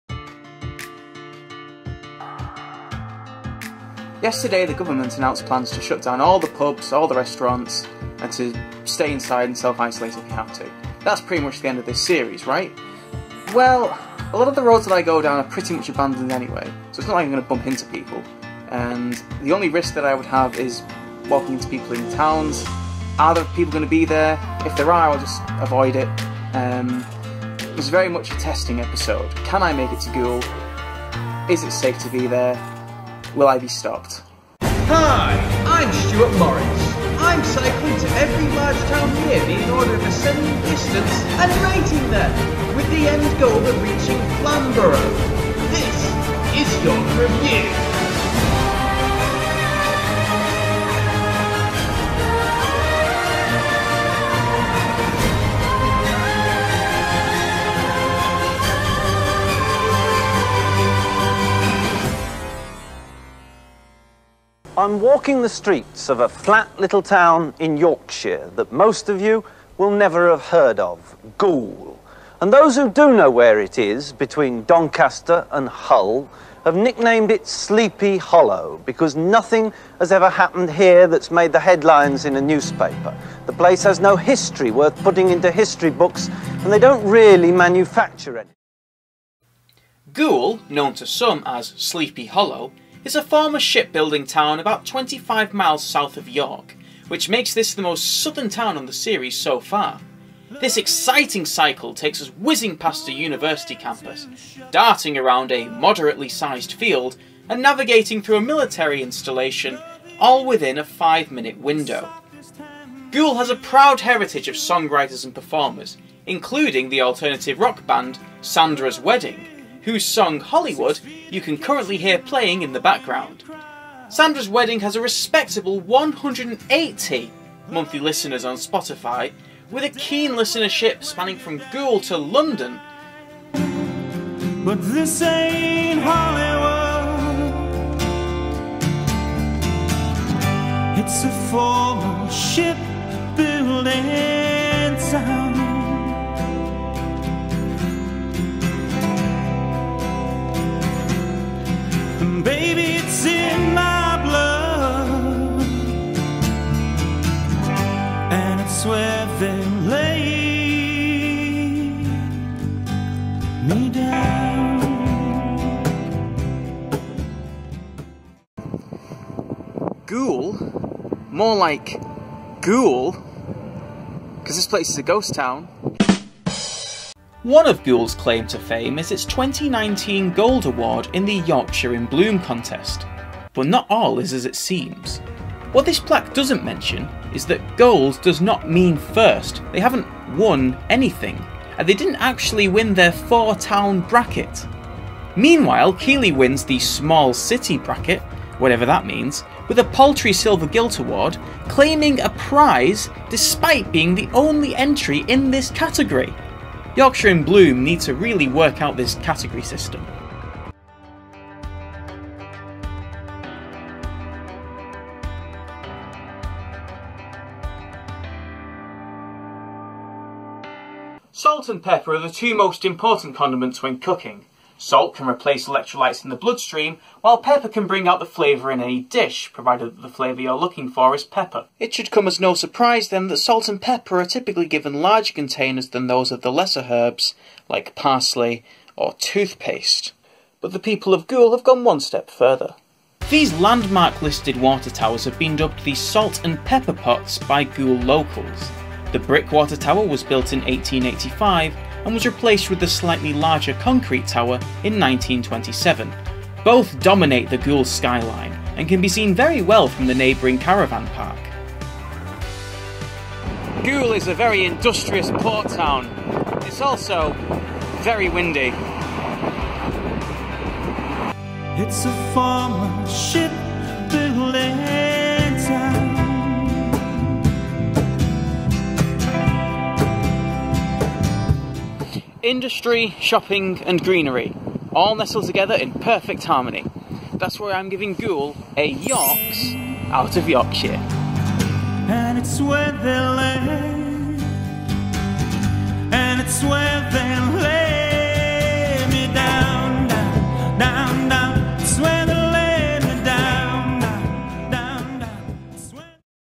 York Review: 9 - Goole
Just when it seemed that everywhere in York was a small market town, we head off to the industrial metropolis of Goole!

I'm cycling to every large place near me in order of ascending distance, and rating them - with the end goal of reaching Flamborough. This is York Review!

This episode was filmed before the official start of the lockdown in the UK. The exercise policy put in place during the lockdown made it clear that York Review could not continue until restrictions were lifted - these restrictions being: "exercise locally", and "no loitering outside (only exercise/shopping)".